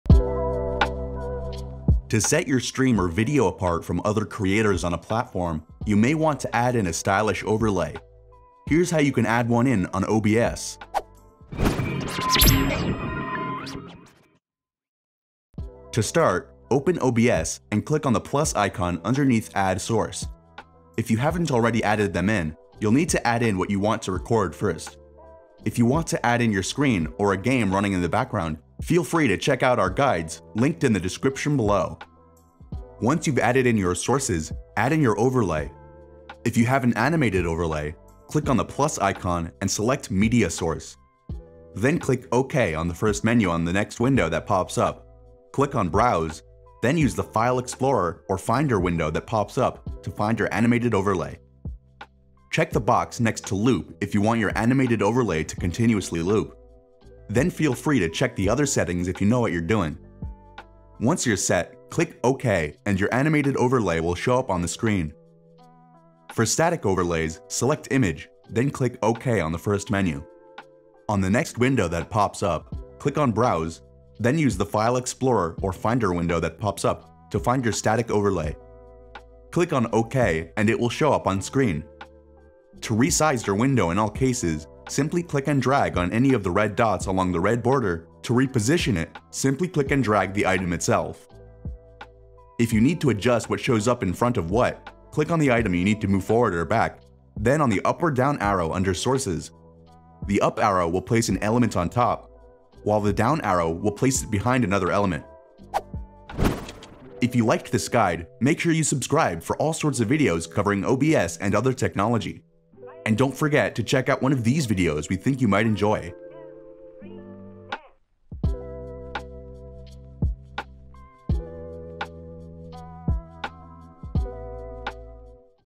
How to Add an Overlay in OBS
To set your stream or video apart from other creators on a platform, you may want to add in a stylish overlay. Here’s how you can add one in on OBS!